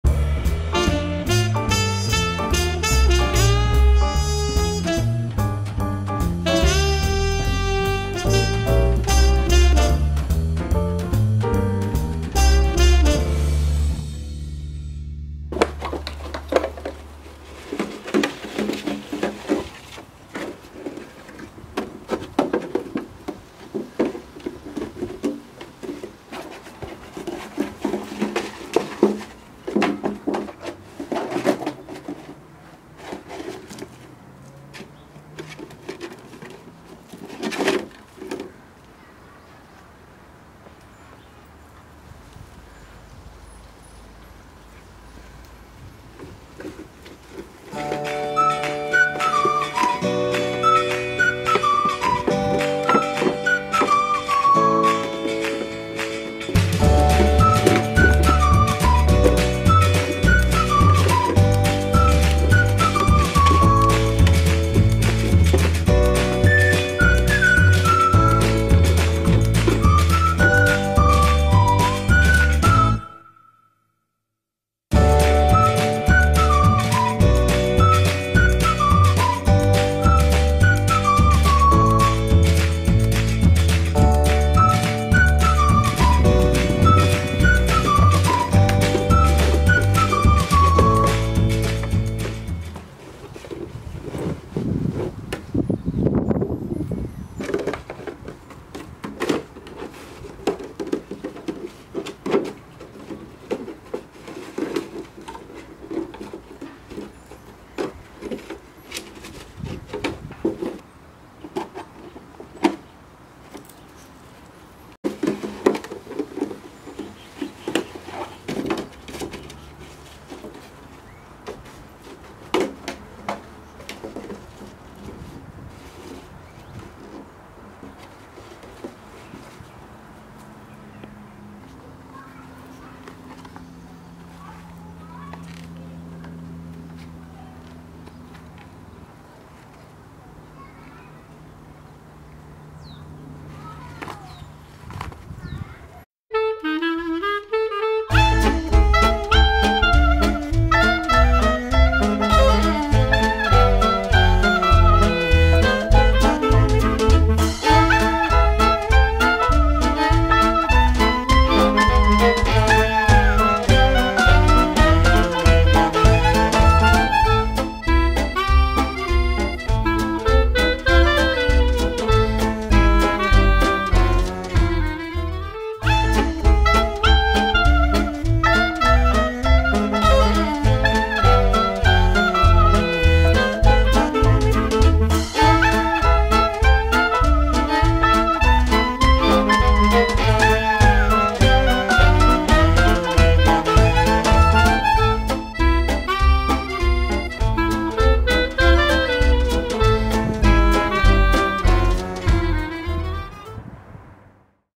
FUN IN THE GARDEN
Fun in the garden and getting stuck in tight spaces!